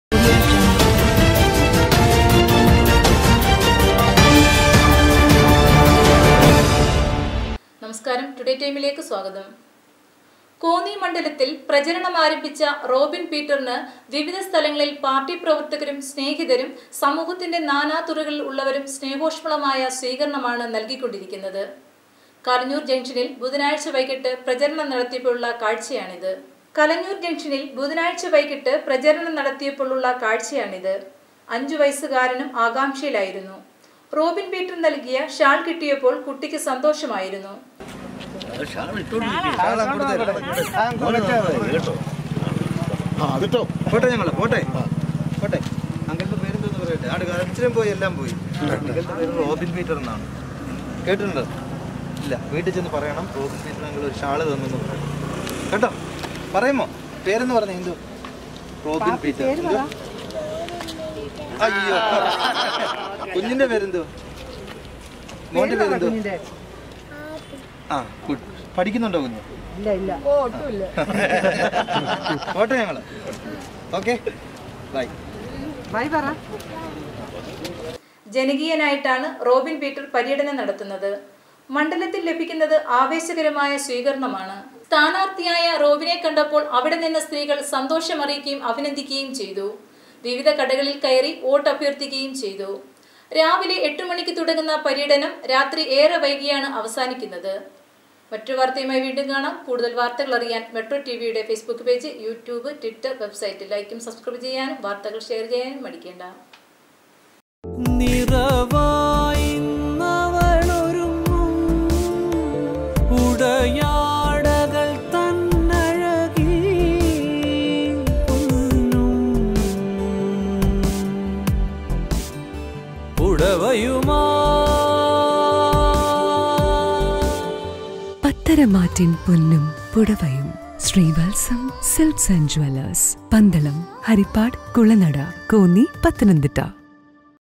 അഞ്ച് വയസ്സുകാരനും ആകാംക്ഷ - റോബിൻ പീറ്റർ നൽകിയ ഷാൾ കിട്ടിയപ്പോൾ സന്തോഷം വീട്ടിൽ ചെന്ന് പറയണം
അഞ്ച് വയസ്സുകാരനും ആകാംക്ഷ - റോബിൻ പീറ്റർ നൽകിയ ഷാൾ കിട്ടിയപ്പോൾ സന്തോഷം വീട്ടിൽ ചെന്ന് പറയണം - ! ജനകീയനായി റോബിൻ പീറ്റർ. മണ്ഡലത്തിൽ ലഭിക്കുന്നത് ആവേശകരമായ സ്വീകരണം